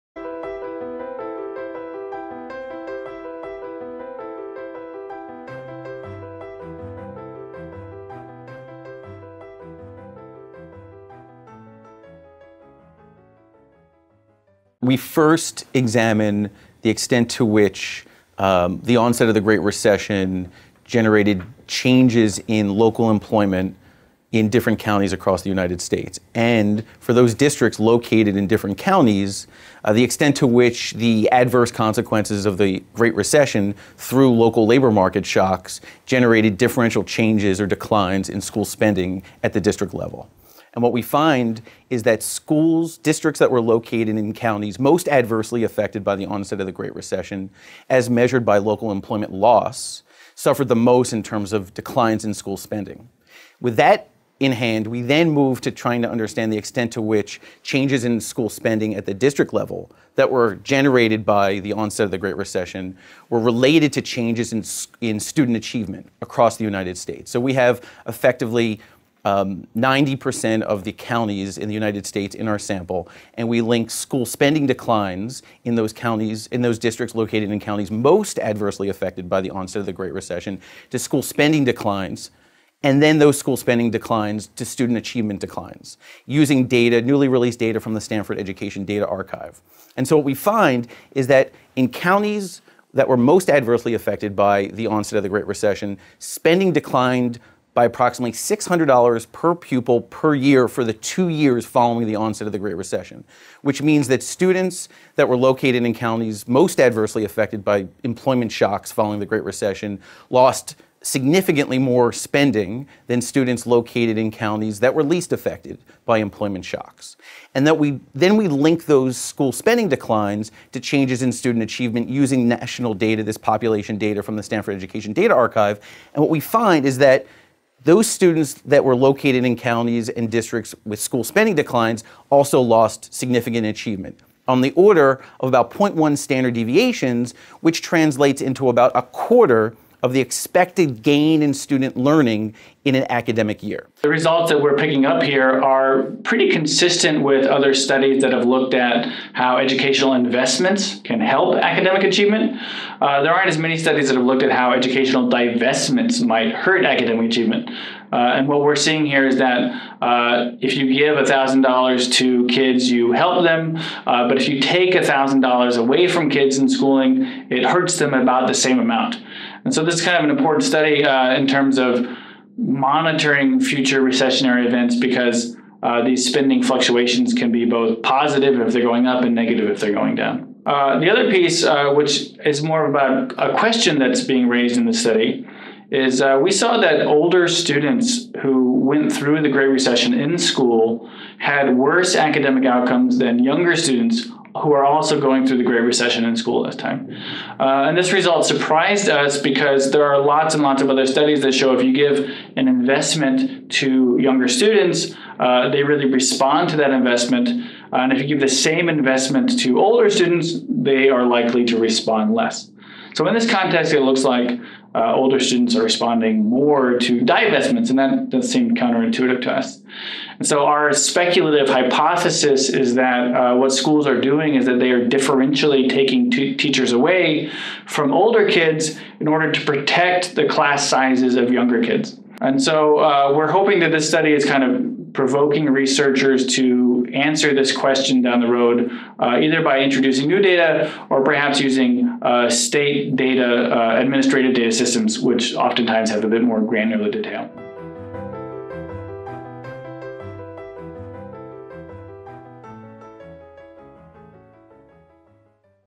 School Spending Cuts Linked to Learning Losses for Students in Hardest Hit Areas
Watch as Matthew Philip Steinberg and Kenneth Shores discuss the findings and implications of a new study published in AERA Open, "Schooling During the Great Recession: Patterns of School Spending and Student Achievement Using Population Data." To learn more or to read the full study, please